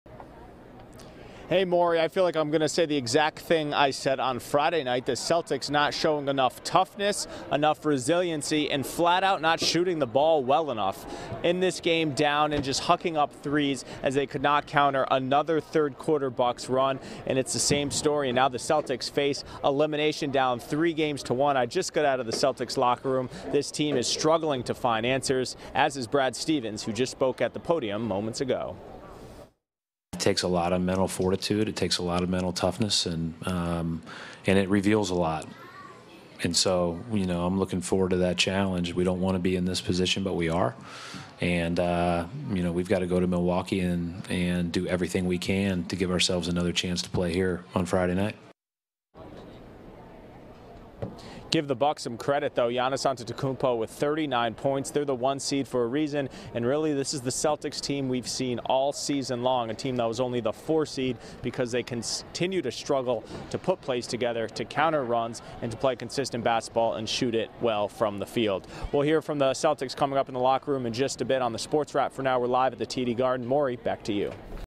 Giannis has 39, Bucks beat Celtics 113-101, lead series 3-1
Giannis Antetokounmpo had 39 points and 16 rebounds to lead the Bucks to a 113-101 victory over the Boston Celtics in Game 4 of their second-round playoff series on Monday night and move Milwaukee within one win from its first trip to the Eastern Conference finals since 2001.